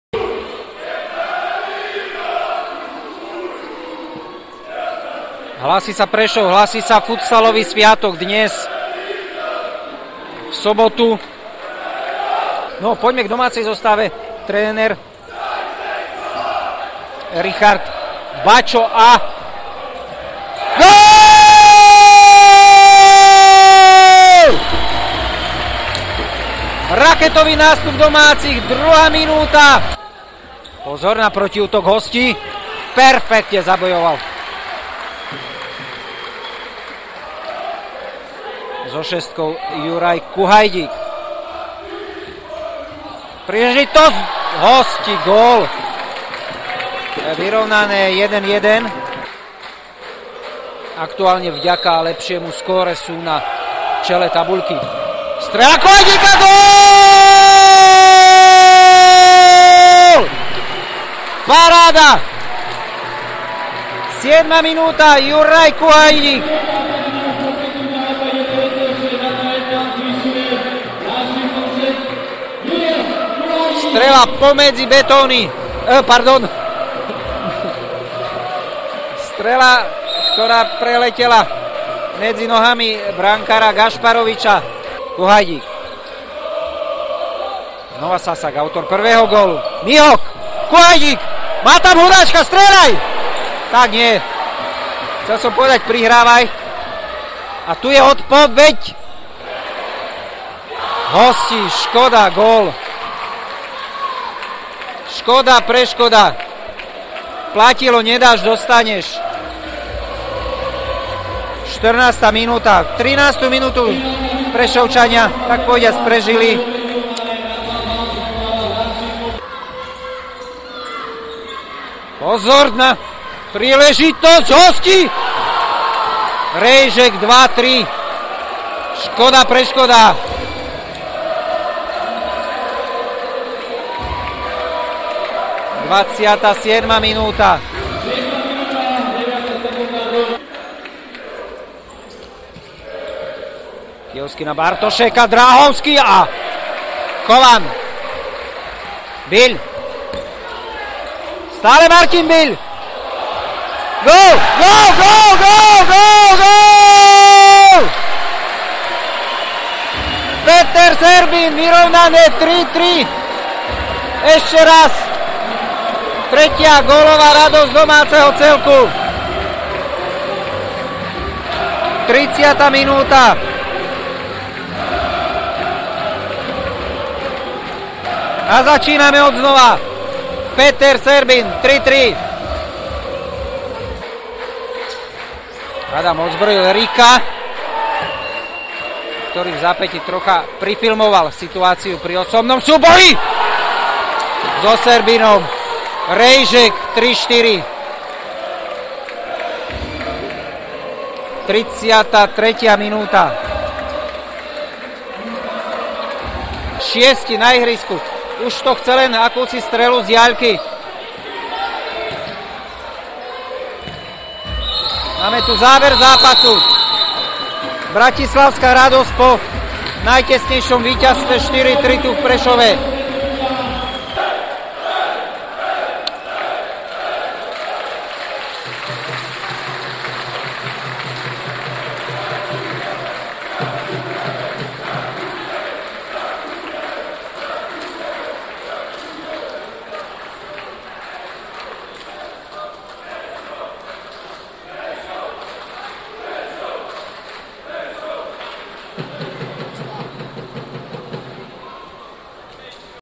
FK Prešov Slov-Matic neskolilo
V 7. kole 1. futsalovej extraligy, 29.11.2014.
FK Prešov - Slov-Matic FOFO Bratislava 3:4 /2:2/
Maroš Sasák   1 : 0
Gabriel Bartošek 1 : 1
Juraj Kuhajdík     2 : 1
Michal Seidler   2 : 2
Martin Rejžek    2 : 3
Peter Serbin     3 : 3
Martin Rejžek    3 : 4